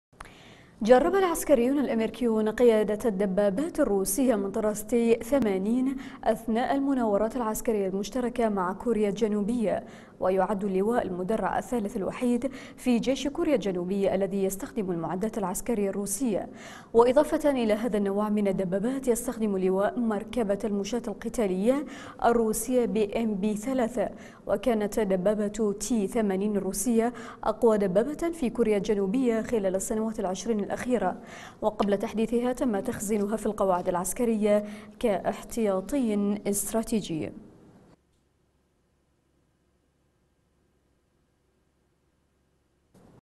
السلاح الروسي العسكريون الأمريكيون جربوا دبابات "تي-80"
السلاح الروسي
العسكريون الأمريكيون جربوا دبابات "تي-80"

جرب العسكريون الأمريكيون قيادة الدبابات الروسية من طراز "تي- ثمانين" أثناء المناورات العسكرية المشتركة مع كوريا الجنوبية. ويعد اللواء المدرع الثالث الوحيد في جيش كوريا الجنوبية الذي يستخدم المعدات العسكرية الروسية. وإضافة إلى هذا النوع من الدبابات  يستخدم اللواء مركبة المشاة القتالية الروسية "بي أم بي- ثلاثة ". وكانت دبابة "تي- ثمانين" الروسية أقوى دبابة في كوريا الجنوبية خلال السنوات العشرين الأخيرة. وقبل تحديثها تم تخزينها في القواعد العسكرية كاحتياطي استراتيجي .